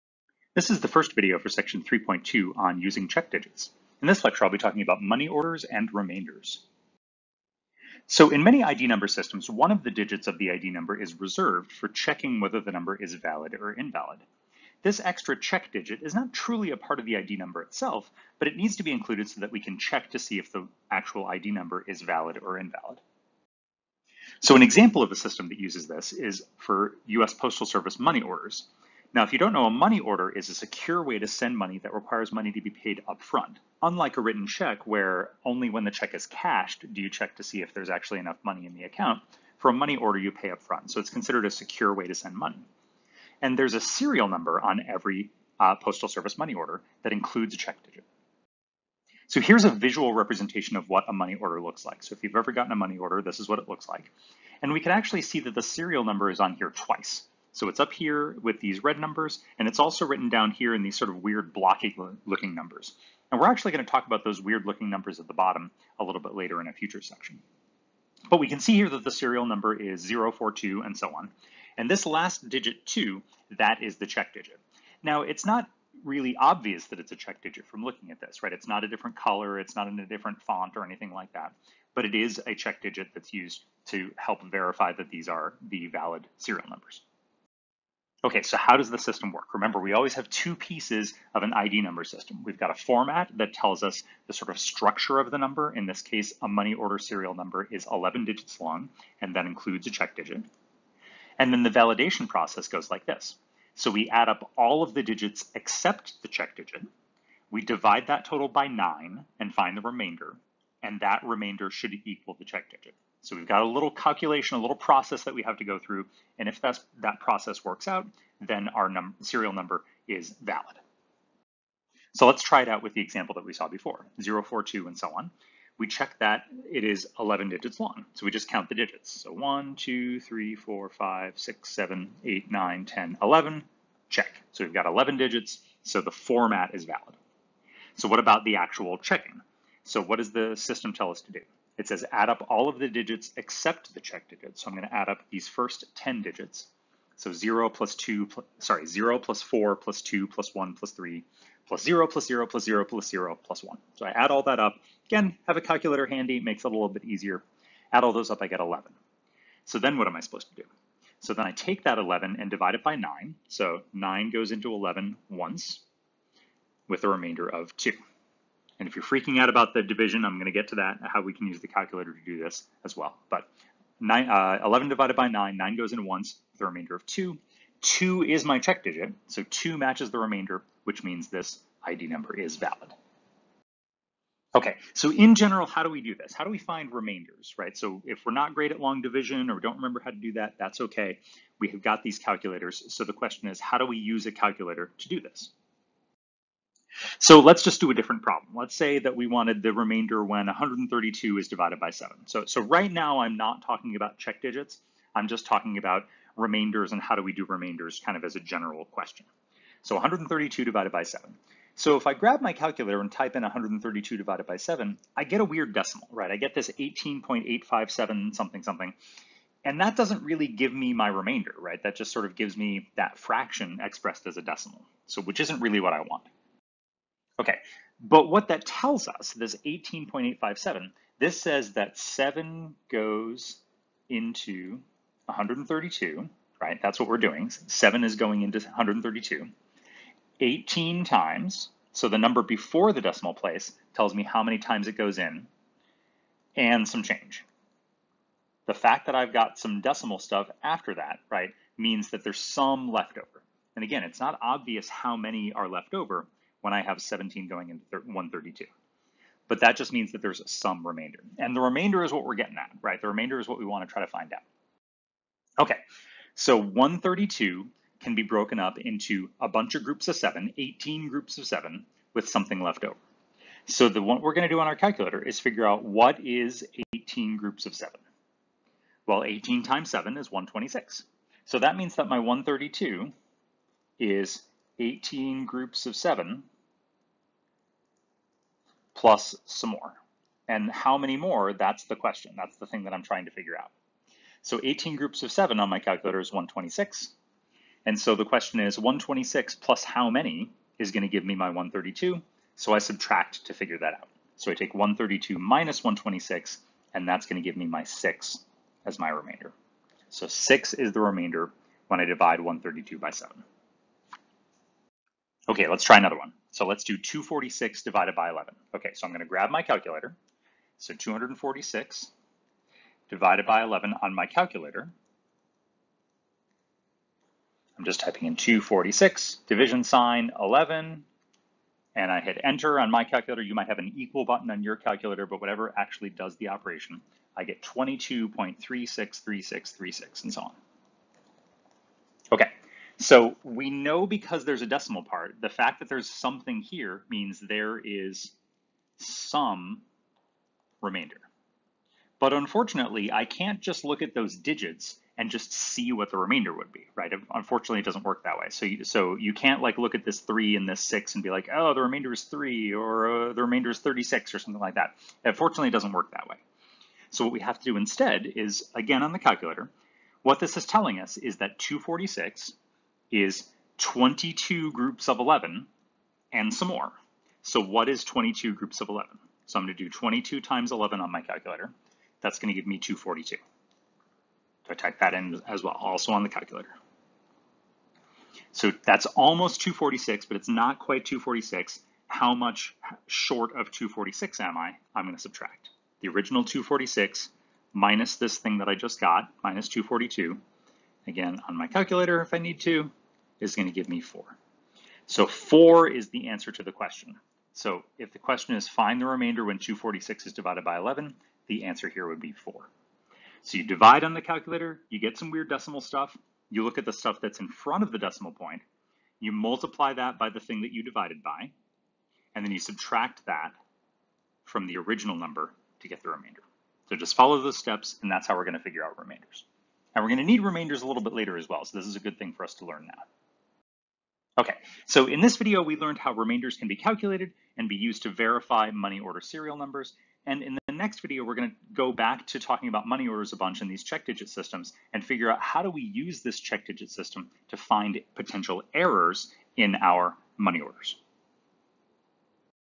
Math for Liberal Studies - Lecture 3.2.1 Money Orders and Remainders
This is the first video lecture for Math for Liberal Studies Section 3.2: Using Check Digits. In this video, we'll focus on the ID number system for U.S. Postal Service money orders. This system uses a check digit that involves the remainder when a number is divided by 9. I explain how to find those remainders on your calculator and work through a couple of examples.